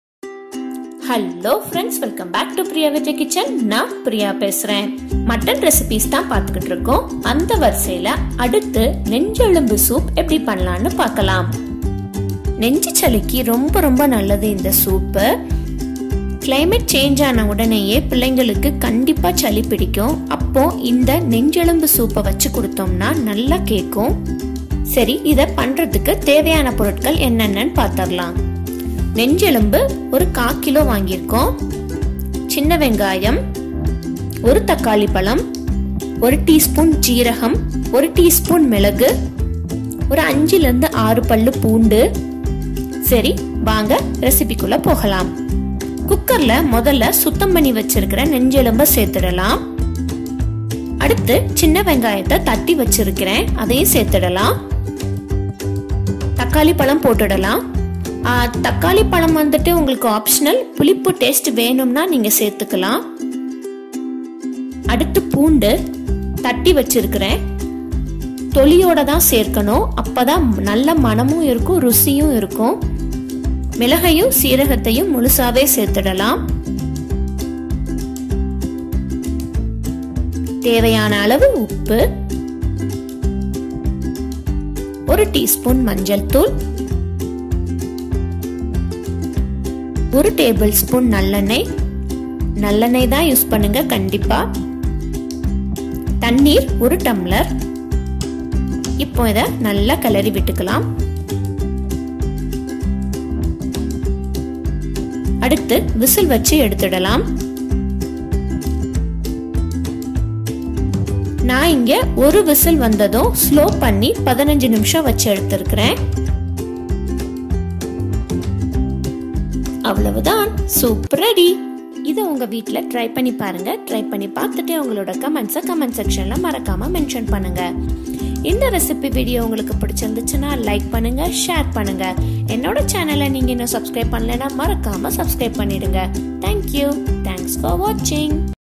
இப்படி செய்தால் 5 நிமிடத்தில் ஹோட்டல் சுவையில் நெஞ்சு எலும்பு சூப் தயார் - nenju elumbu soup
Next in Mutton recipes is Nenju Elumbu Soup”
Today let us see how to prepare Nenju Elumbu Soup. Nenju elumbu soup is very good remedy for cold and cough. During climate change, it is recommended to prepare nenju elumbu soup and make your kids drink.
Let’s start!
Ingredients needed:
Nenju Elumbu – 250g
Shallots – 10 to 12
Tomato – 1
Pepper corn – 1 tsp.
Cumin seeds – 1 tsp.
Garlic Pods -5 to 6
In cooker, add nenju elumbu (Goat Rib bone)
In a mixer grinder add shallots and whip it once or twice
Add whipped shallots
Next add tomato. Tomato is completely optional.
Add crushed garlic
Then add whole pepper corns and cumin seeds
Add required salt
Next add 1tsp. of turmeric powder
Add 1 tbsp. of gingerly oil
Add 1 cup of water
Cook for 5 whistles or after 1st whistle, slow down the flame and cook for next 10 minutes
Soup ready !